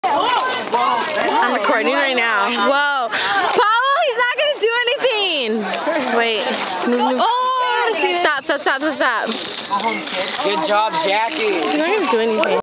Preston Vs Jackie/Paul
Paul Was Making Fun Of Preston And He Tried 2 Act All Bad Then They Startd Fighting. Paul Didnt Wanna Fight Him Though Cause Hes Little. Jackie Tried To Break It Up But Ended Up Fighting Him. I Know Its Really Short And Barely A Fight